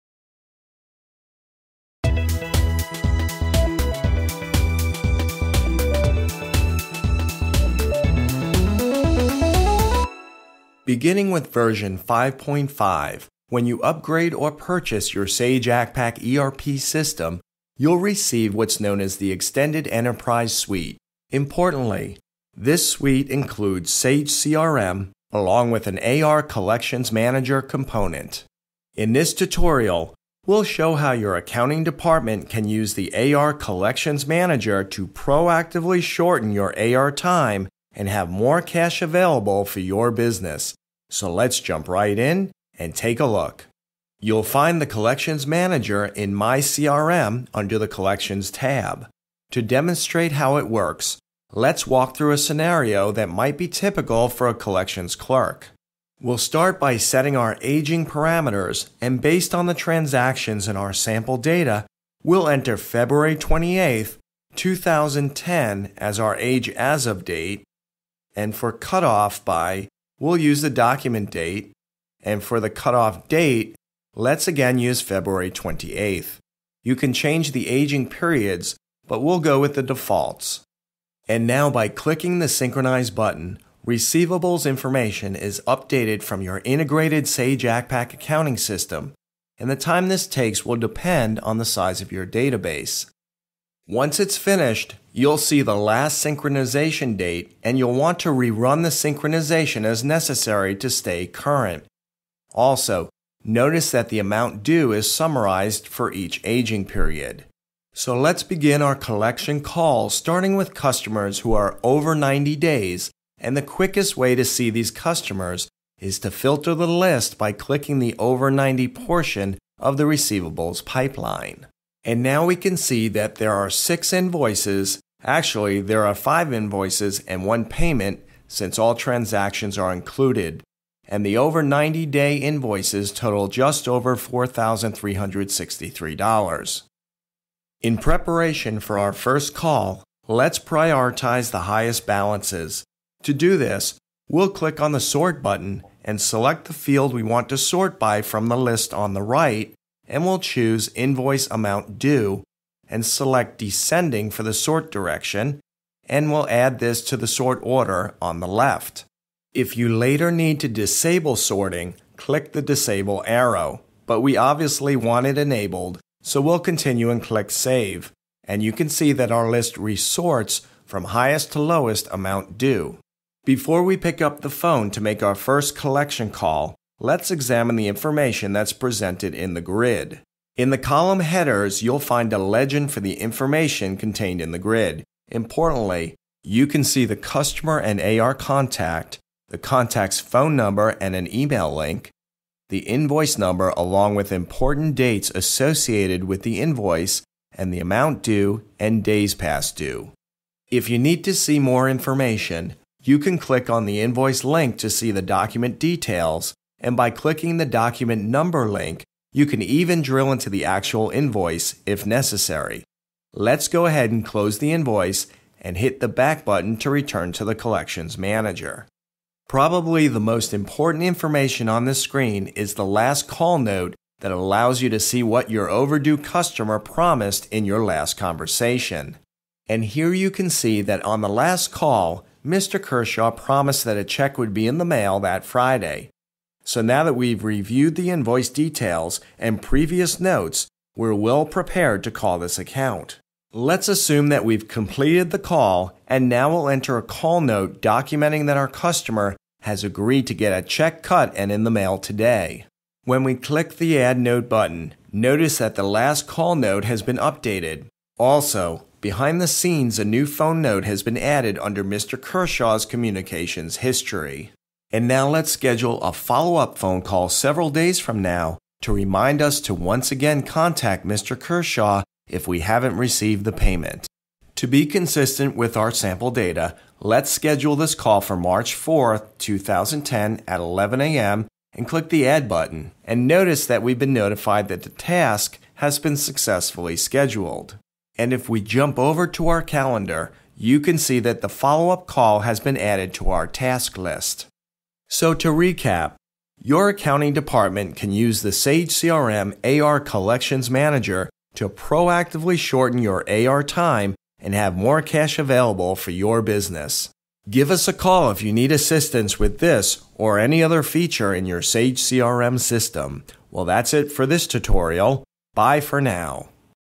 Sage 300 ERP AR Collections Manager
The Sage Accpac ERP Extended Enterprise Suite now includes the AR Collections manager. AR Collections manager consolidates customer information from many areas within Sage Accpac putting valuable information at the fingertips of your staff. In this tutorial, we lead you on a tour of this module.